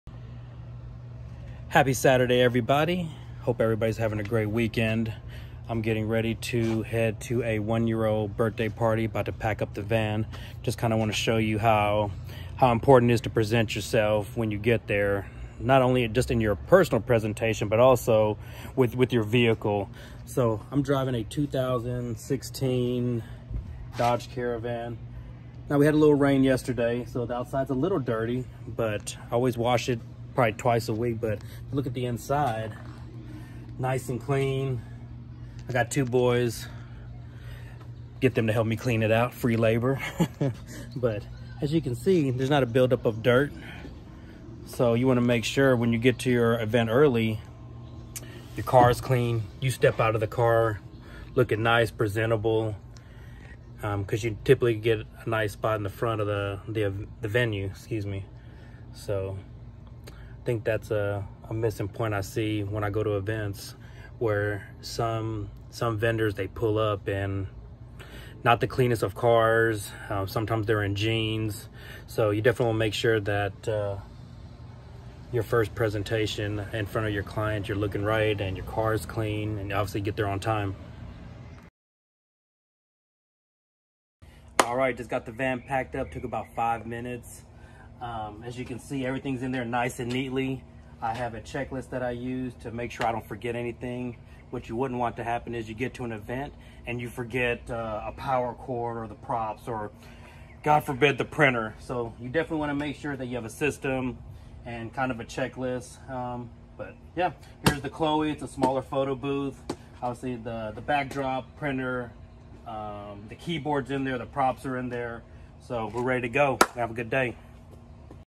What to expect when you book a photo booth from Flash Party Photo Booth.
Today I’m sharing behind the scenes on how I prepare and pack my van for an event. I use a check list that is in my garage and have my boys help me load everything in the van. I have all my equipment and props in exact areas. Being organized and having a process will help you ensure you don’t forget anything. As I mentioned in the video when you arrive to the venue. Taking out your equipment will be easy.